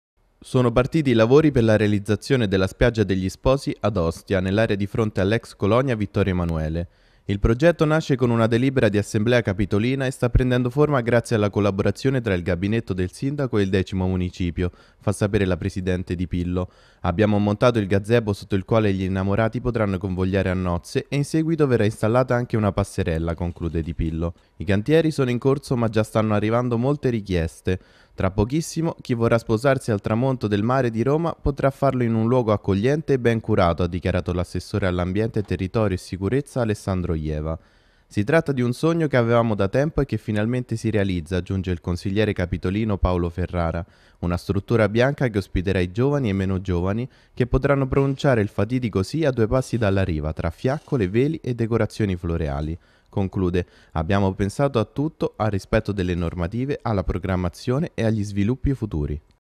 Ostia: spiaggia degli sposi, lavori in corso - Canale 10
Lavori in corso sulla spiaggia degli sposi ad ostia di fronte all'ex colonia Vittorio Emanuele.  A breve sara' ultimata la realizzazione della passerella.